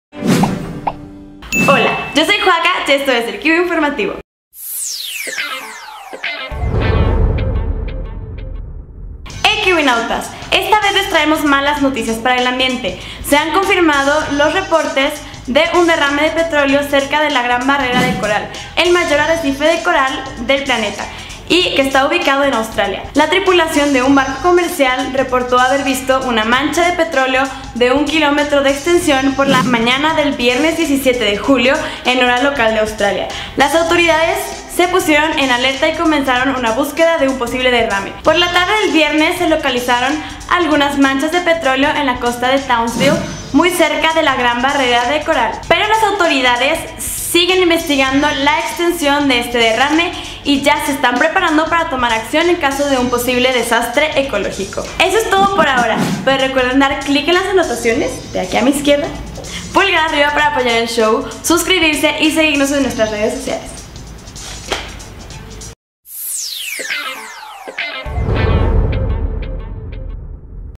Derrame de petróleo en La Gran Barrera de Coral
Malas noticias para el ambiente, se registra un derrame de petróleo cerca del arrecife de coral más grande.

Créditos:

Conducción:
Joaca / @joaquinaan

Escrito por:
Carlos Torres

Producción:
Fernando Díaz

Postproducción:
Daniel Rojas